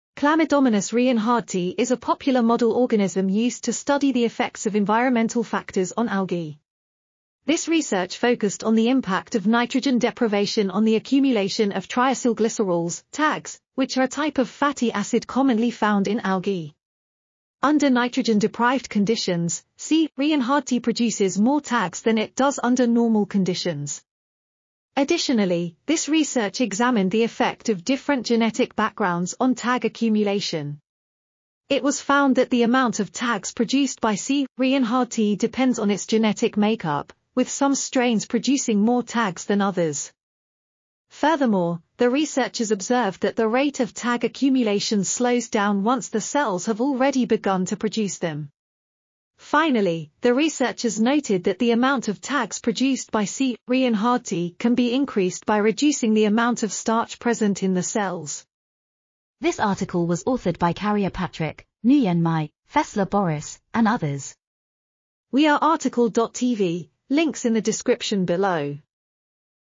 Oil accumulation in the model green alga Chlamydomonas reinhardtii: characterization,... | RTCL.TV
### Keywords ###

### Article Attribution ###
Title: Oil accumulation in the model green alga Chlamydomonas reinhardtii: characterization, variability between common laboratory strains and relationship with starch reserves
Authors: Carrier Patrick, Nguyen Mai, Fessler Boris, Cagnon Caroline, Cuiné Stéphan, Siaut Magali, Beyly Audrey, Beisson Fred, Triantaphylidès Christian, Li-Beisson Yonghua ,and Peltier Gilles
Publisher: BMC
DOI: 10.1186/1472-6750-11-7

### Video Timestamps ###
0:00:00 - Summary
0:00:59 - Title
0:01:05 - Outro
0:01:09 - End